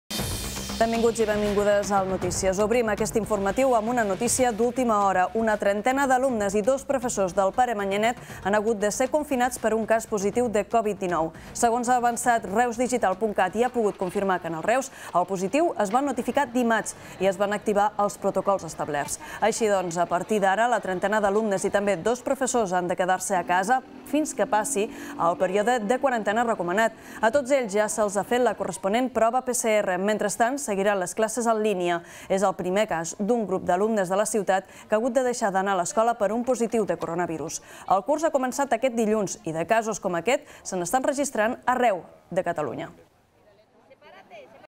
Una trentena d'alumnes confinats al Pare Manyanet de Reus
Es tracta d'un dels cursos de quart d'ESO de l'escola reusenca. S'hi hauria notificat un cas positiu de COVID-19 que ha obligat a confinar els alumnes i també dos professors.